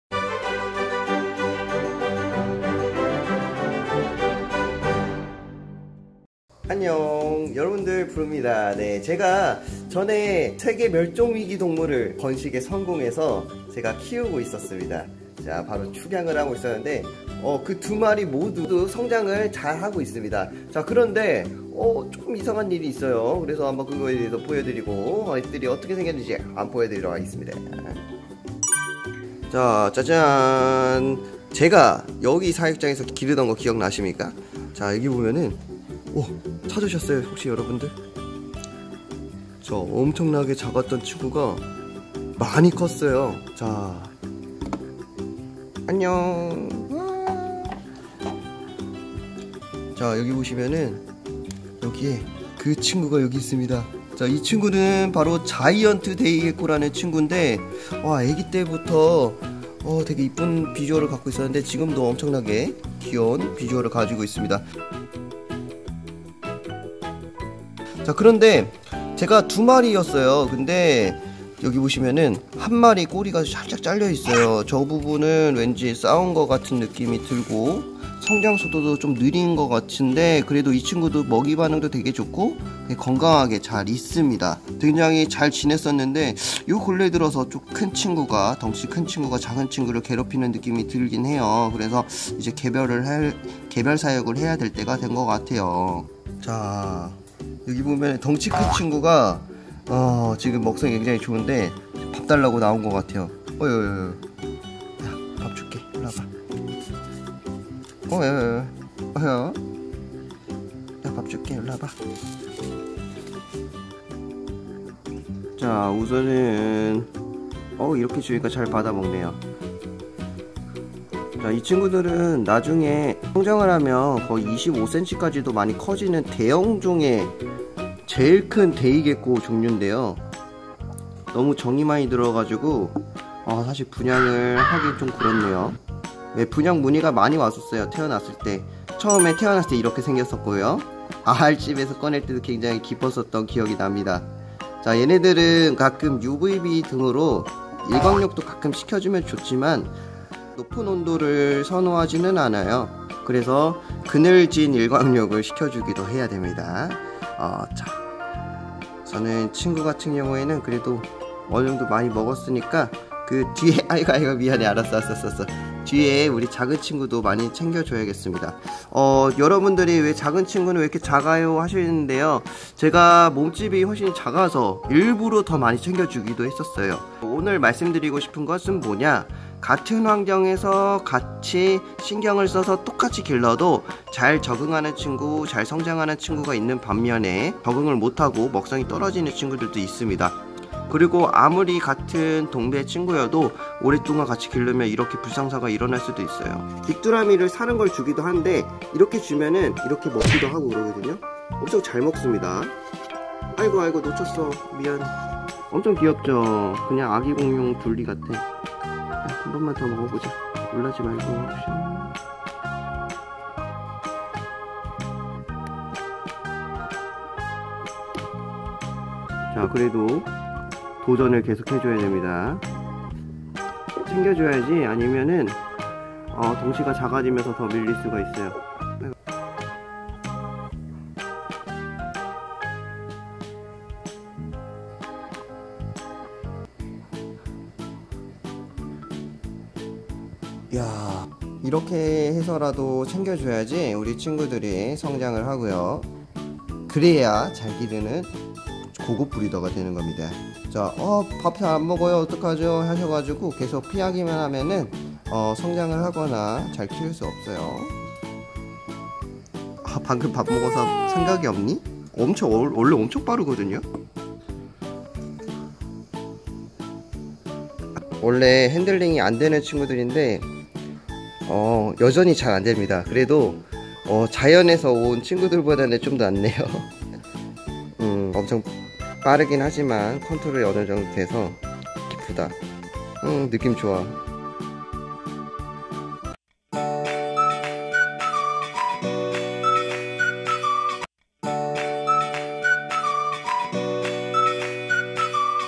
세계멸종위기종 "번식성공"이후 이렇게 되었어요~!! 아프리카 가고싶다!![정브르]
데이게코들은 정말 관상용으로도 좋고 키우면서 산란도 받기 좋은 종인데요.  데이게코중에서는 제일 대형급인데요~!
그 다음으로는 스탠딩 데이게코가 대형급입니다!
열대성과일을 좋아하고 생김새가 귀엽고 브리더분들에게 인기가 많다보니 포획이 많아져서   아프리카~ 마다가스카르 섬 전역에서 볼수있었던 데이게코가 지금은 많이 줄어들었다고 하네요!

최근 하와이에서는 피콕데이게코도 보았다는 사진도 올라오고 하는데요! 직접 나중에 채집을 가보고 싶네요^^

해외채집은 항상 두근두근 거립니다~~